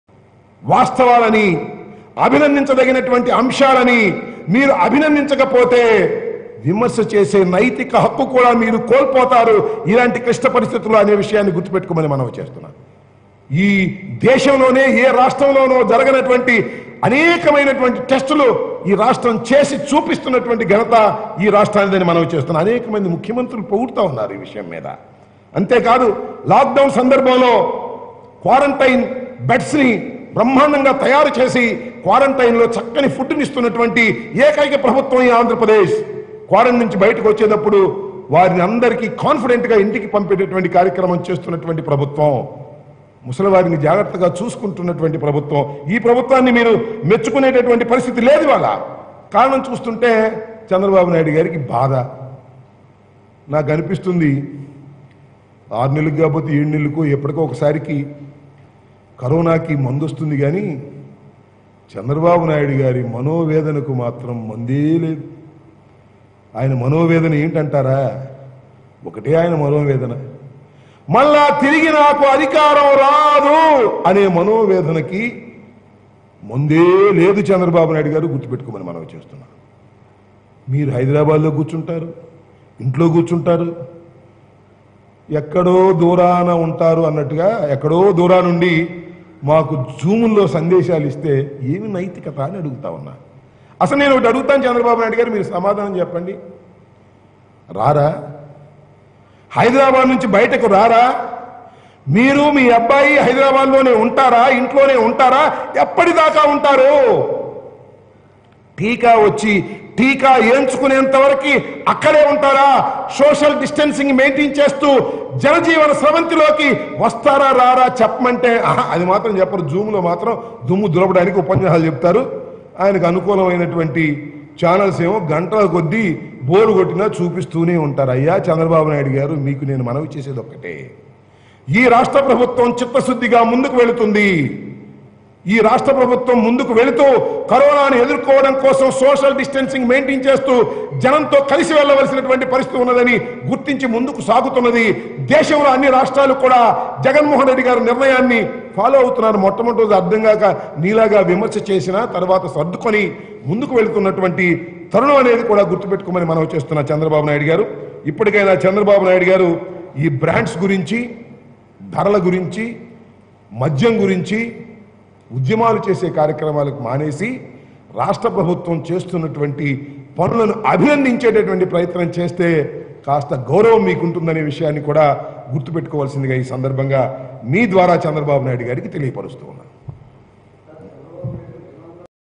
Ambati Rambabu Strong Comments on Chandrababu over Coronavirus | MOP
Ambati Rambabu Strong Comments on Chandrababu over Coronavirus